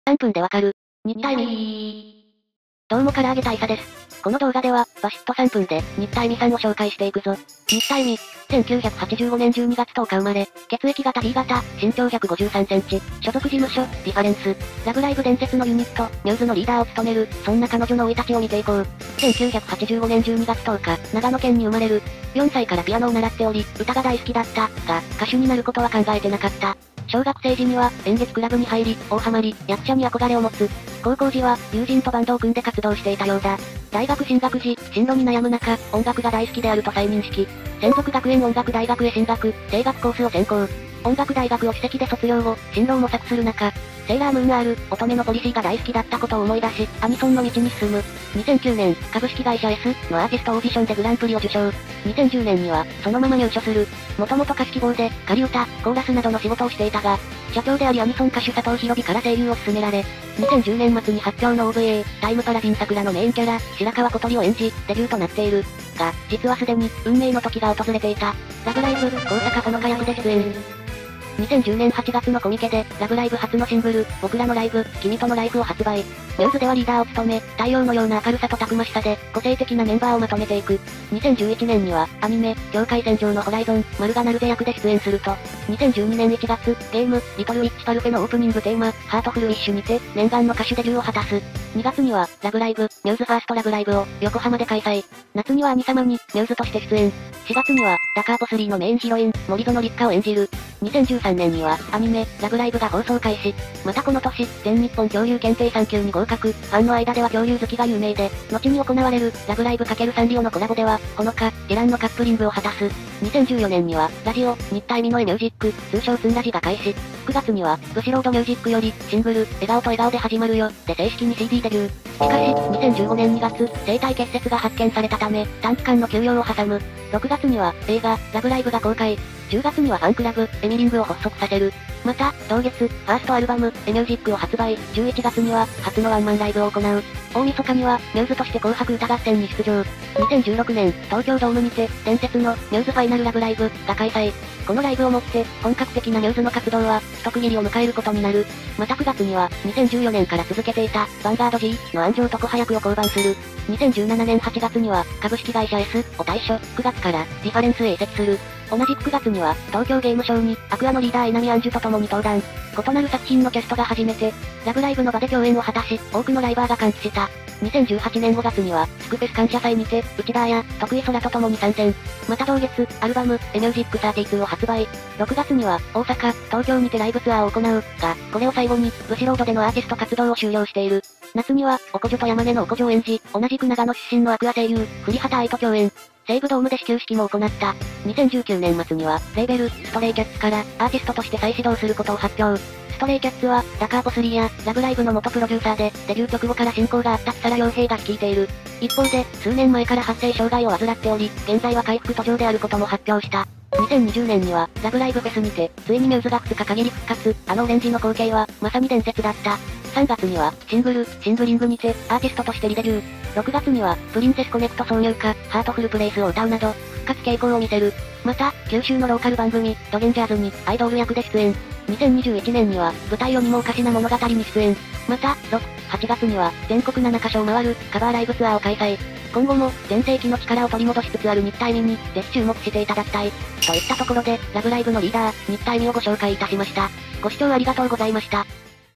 3分でわかる！新田恵海
ラブライブ！μ’sのリーダー『高坂穂乃果　役』えみつん　こと新田恵海さんの紹介です

親と喧嘩して、裸足で屋根を伝って逃走するえみつん。実家の壁に穴をあけてしまうえみつん。それでも、μ’sのリーダーはこの人以外ありえない！